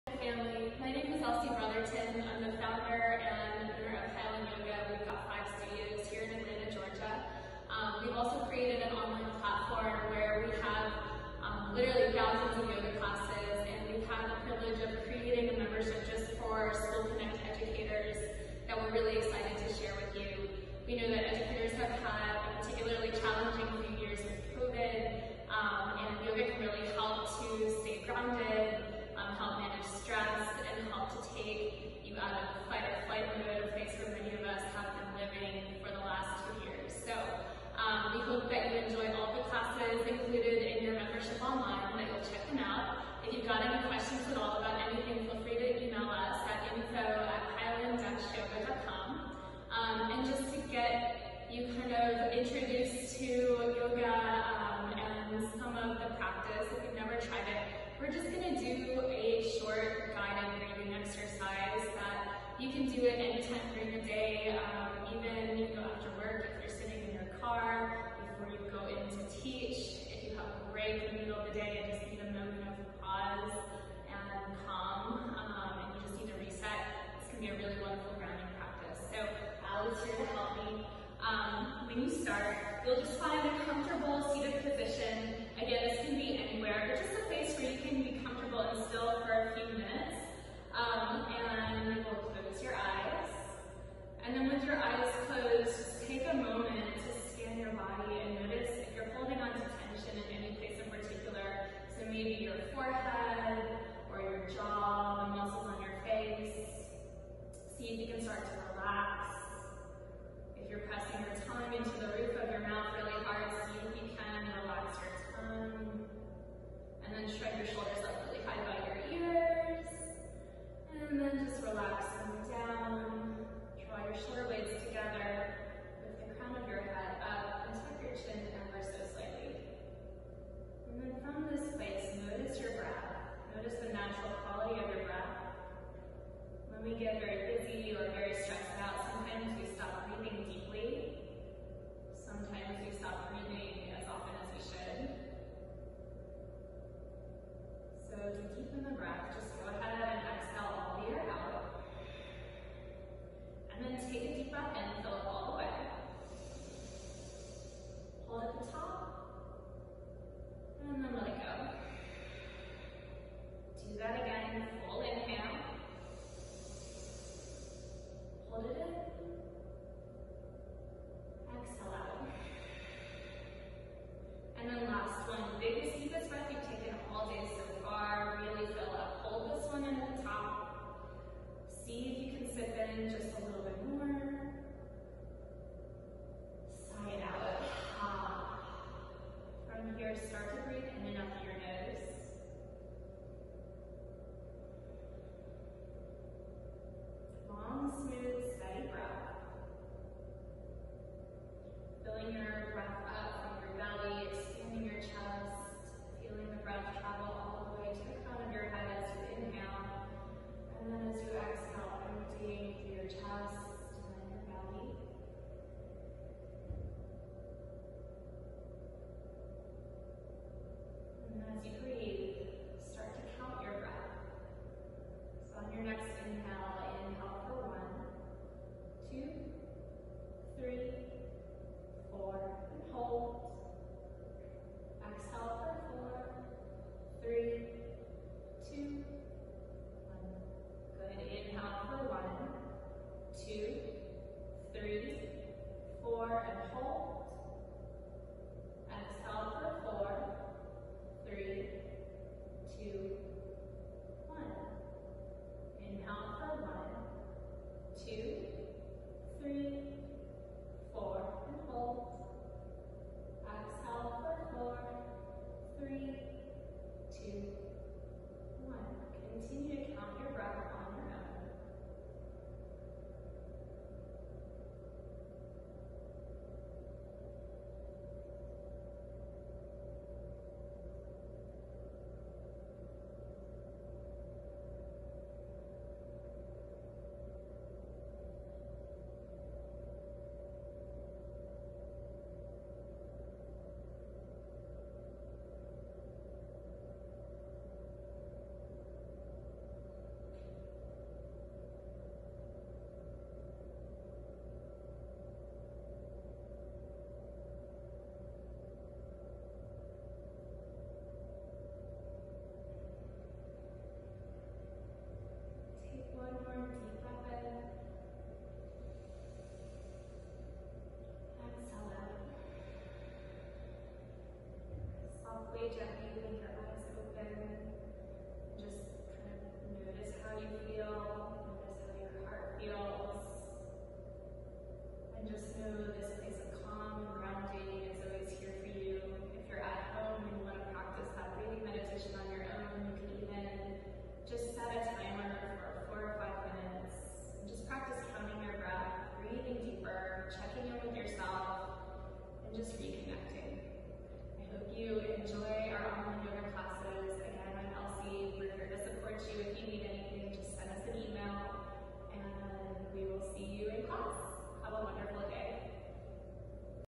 Free Online Highland Yoga for Educators
This is a free online subscription to Yoga Classes from Highland Yoga! Take classes at home or with fellow educators at school. Password is: SCHOOLCONNECT